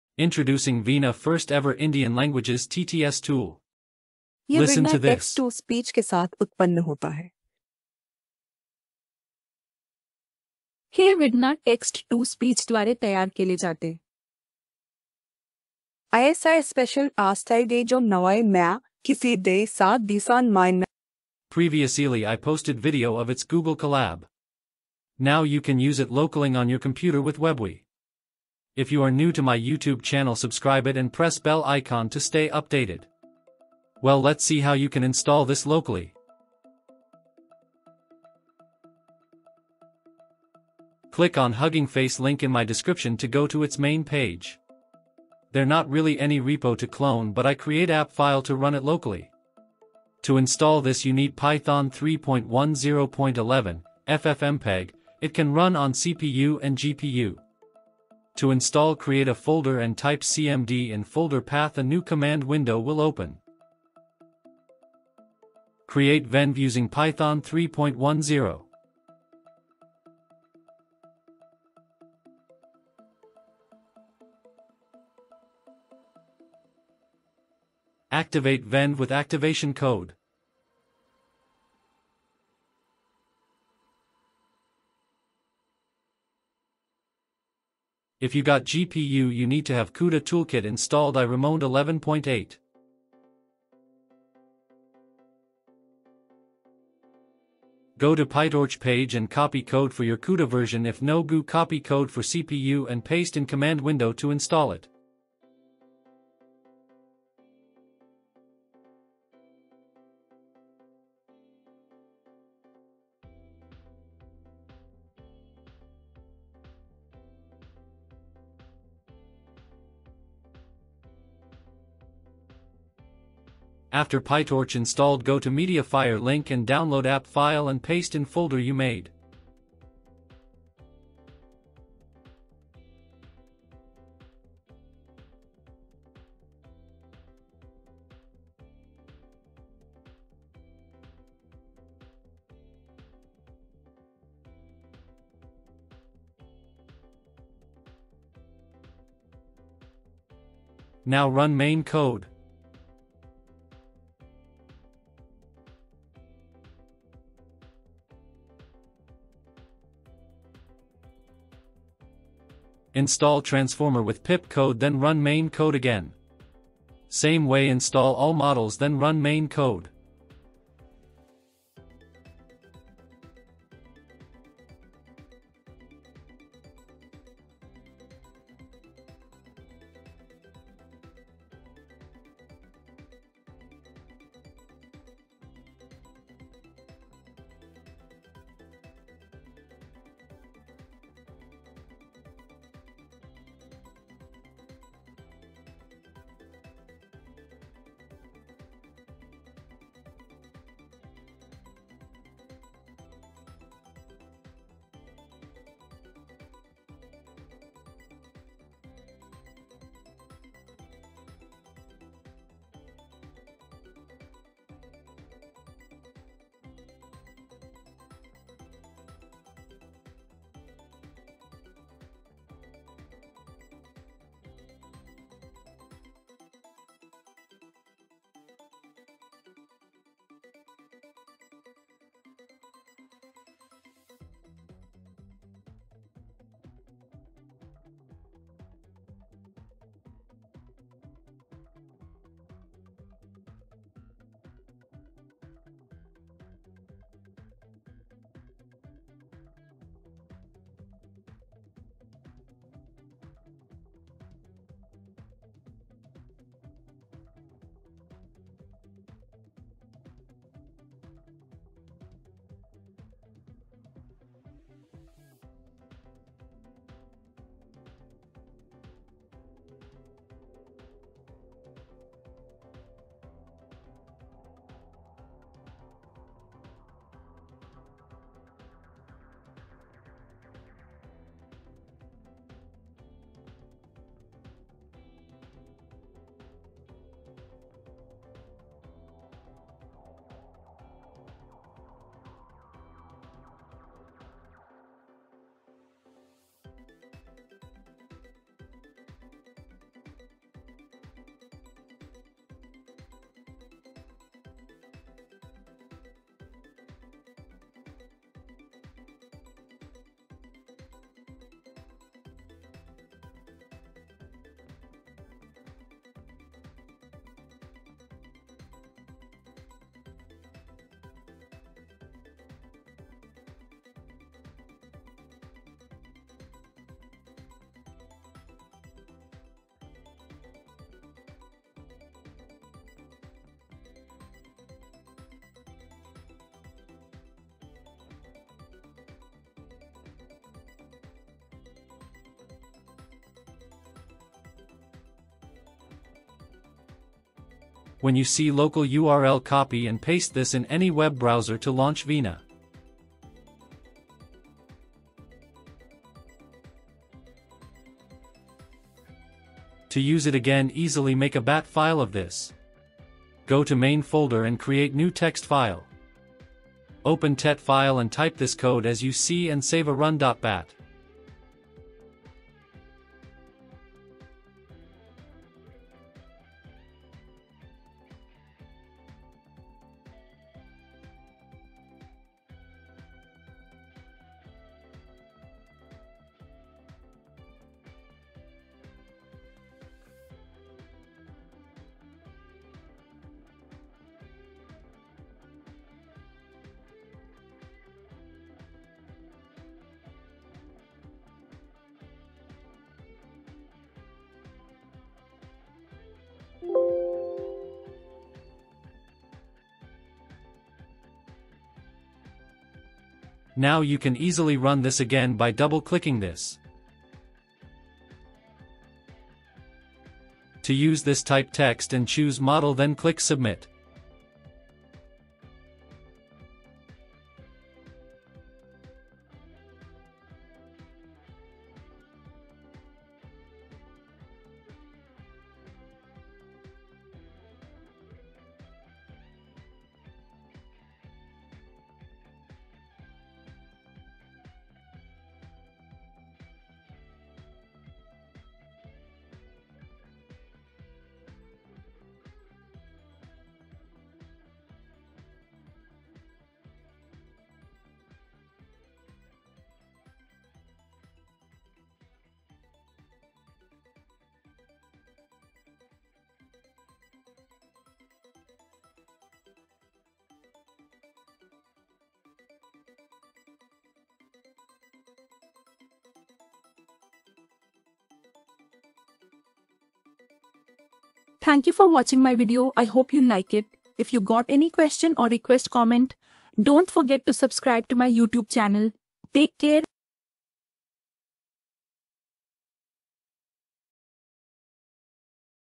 "Install Veena TTS Locally with WebUI | Realistic Indian Voice AI on Your PC"
Run Veena TTS locally with a WebUI and experience the first-ever Indian text-to-speech model that supports Hindi, English, and Hinglish!
This powerful voice AI by Maya Research generates realistic Indian voices with ease — perfect for voiceovers, narration, or any Indian language project.

✨ Features:

Fully offline local setup

Gradio-based WebUI

Works with GPU or CPU (slower)

4 Indian-style voices: Kavya, Agastya, Maitri, Vinaya

Converts Hindi + English text into natural speech